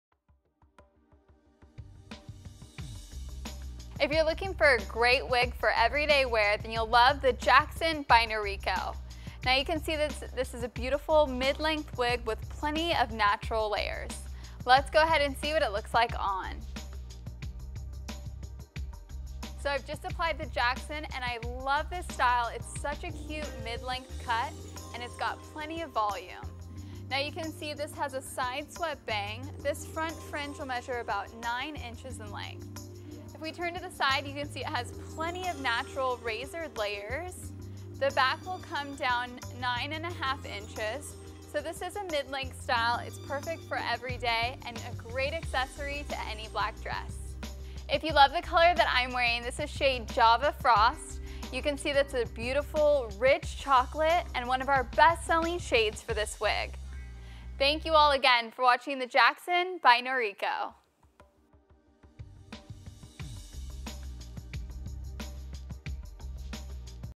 Noriko Jackson Wig Review
You will love this sleek sensation! Jackson by Noriko wigs is a smoldering, casual cut with softly disconnected layers and lightly amplified tresses.